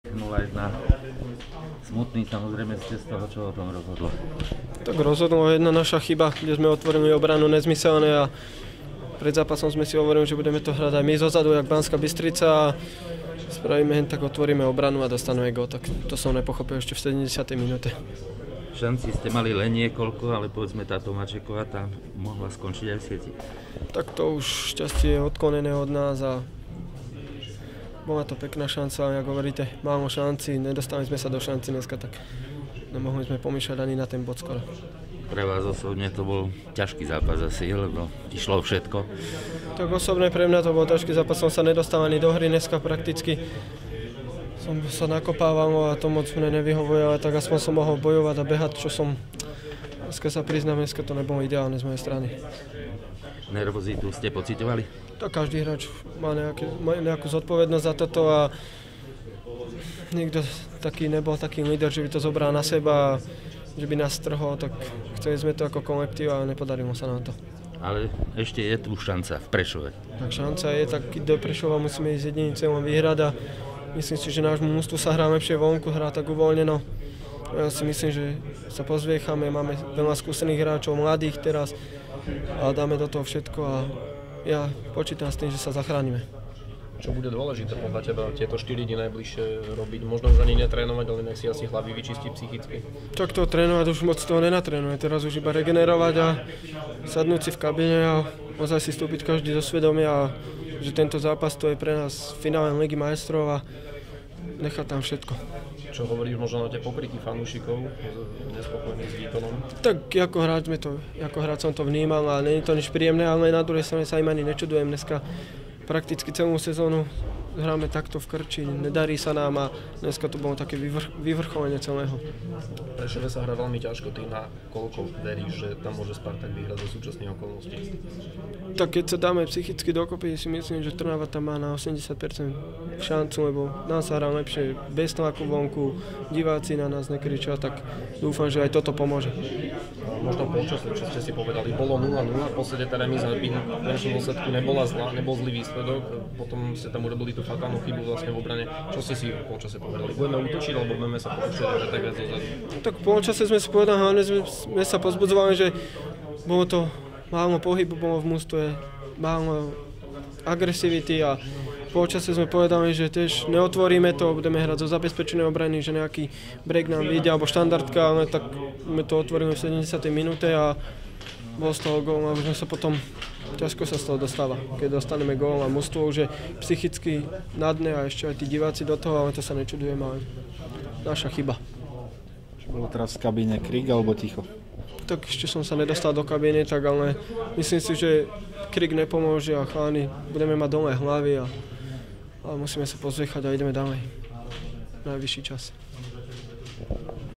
Martin Mikovič po zápase Spartak - Banská Bystrica 0:1 (0:0)
Martin Mikovič (Spartak) hovorí o zápase s Banskou Bystricou.